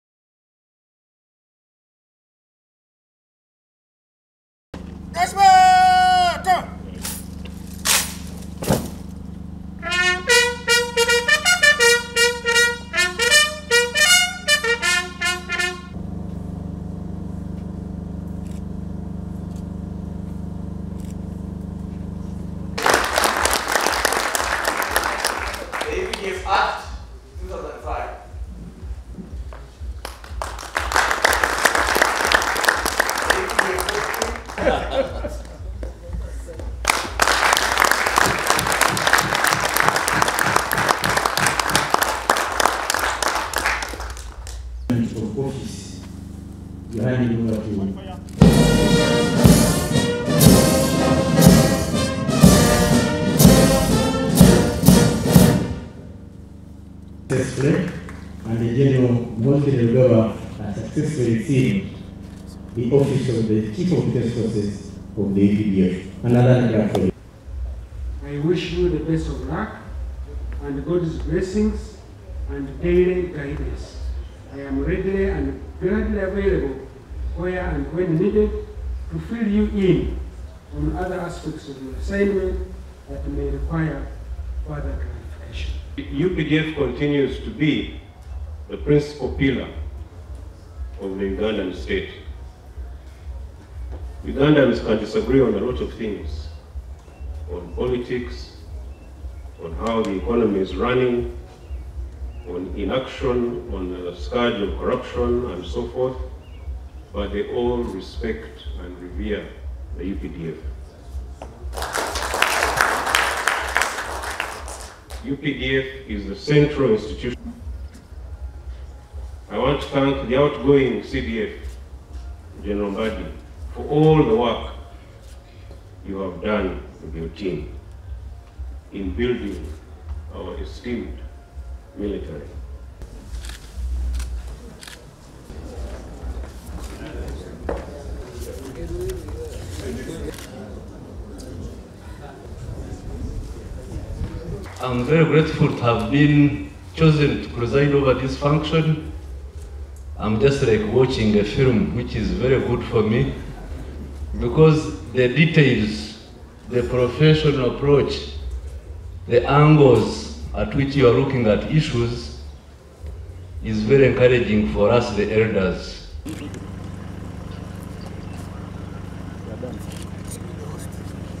Gen Muhoozi Kainerugaba is the 13th Commander of UPDF
Gen Mbasu Mbadi  hand over to Gen Muhoozi Kainerugaba as the 13 commander defence forces of updf at the function presided over by Gen Salim saleh